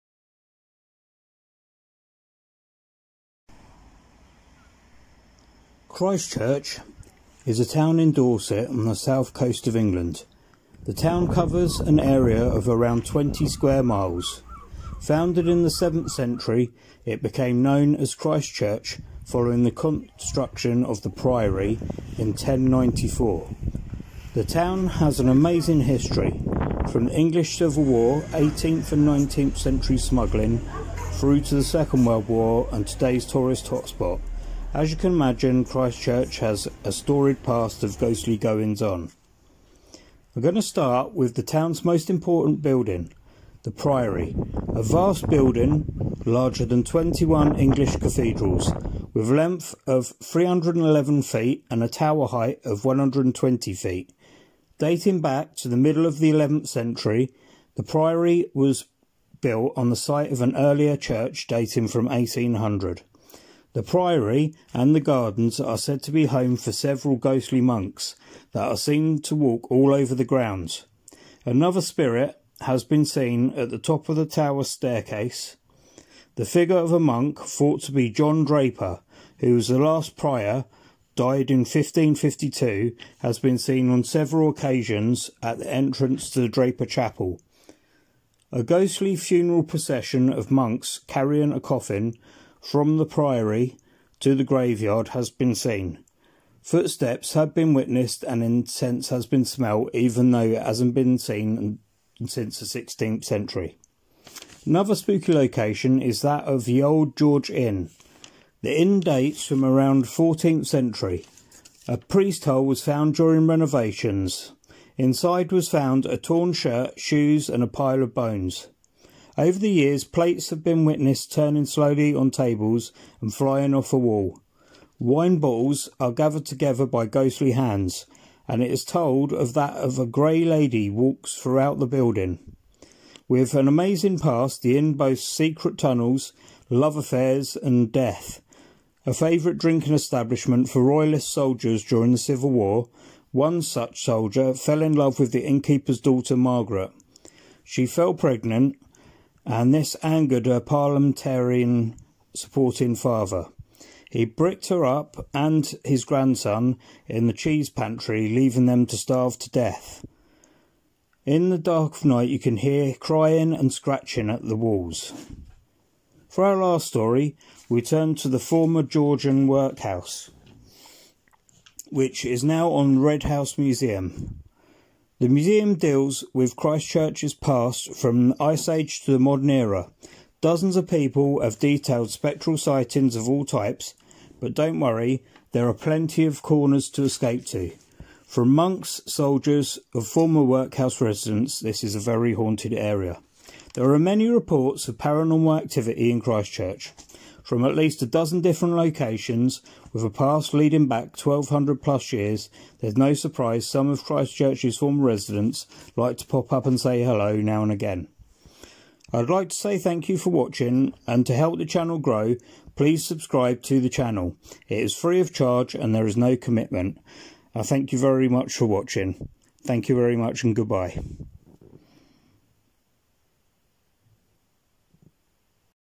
GHOSTS OF CHRISTCHURCH
Here we have a short history of ghosts of Christchurch.